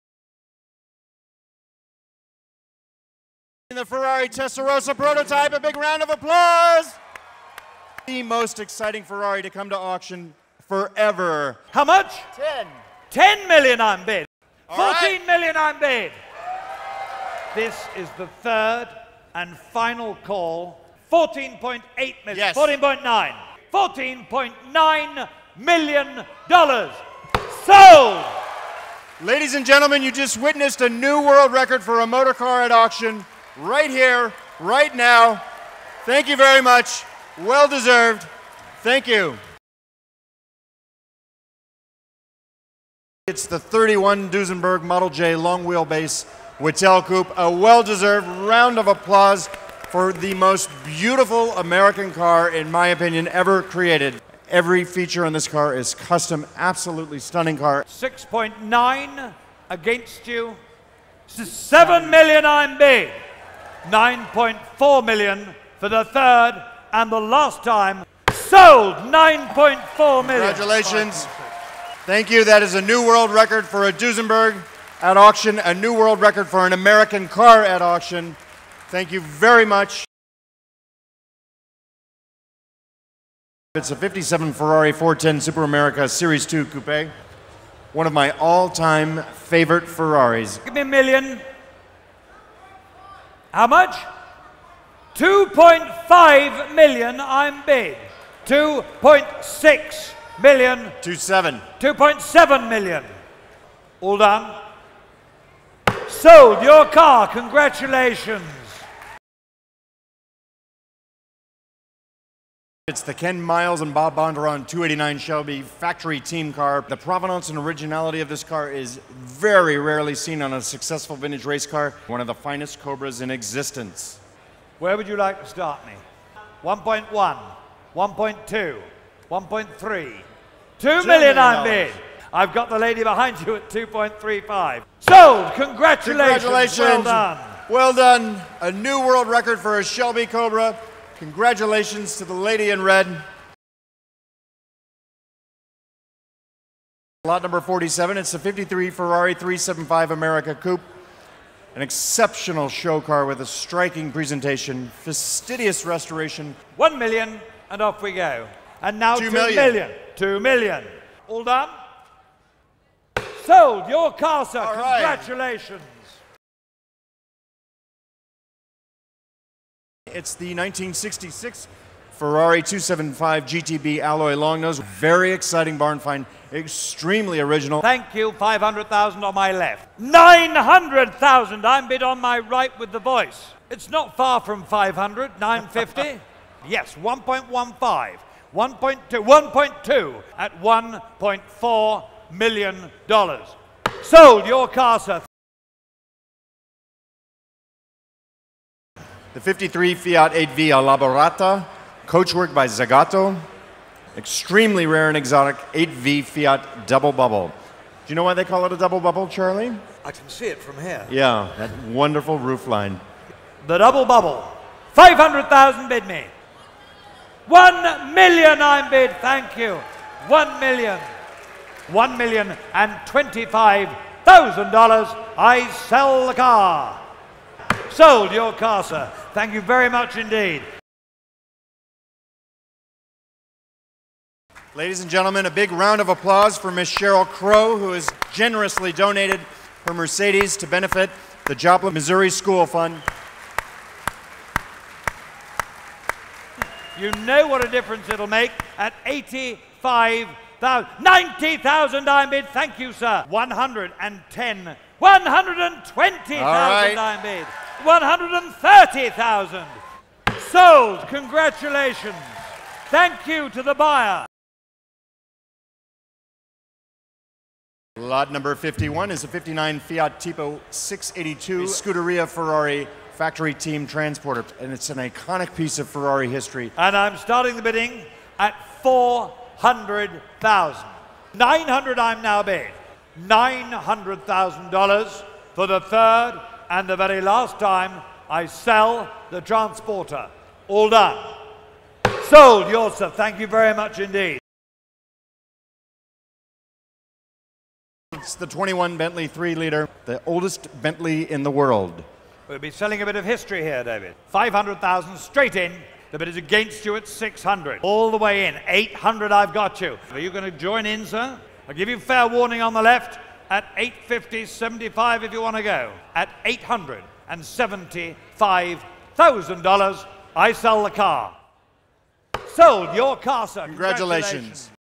Gooding & Company - 2011 Pebble Beach Auction Top Sales and Highlights
Check out Gooding & Company's top sales and highlights at the 2011 Pebble Beach Auction.